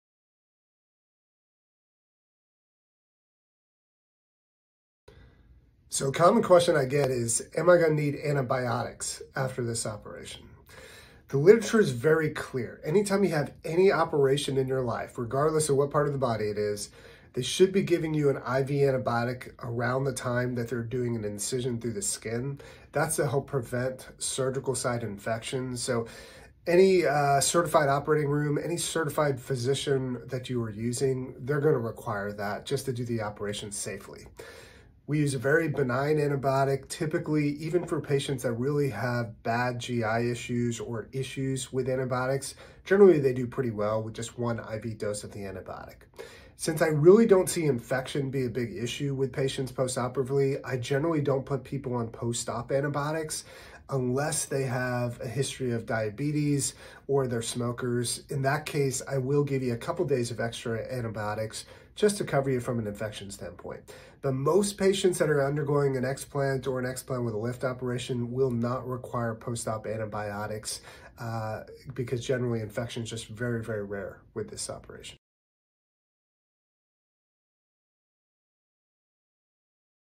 Will you need antibiotics after your explant procedure?
Generally you will get a dose of IV antibiotics at the time of surgery. This is to prevent surgical site infections. Infection is really rare with these operations so generally I don't put patients on post op antibiotics by mouth.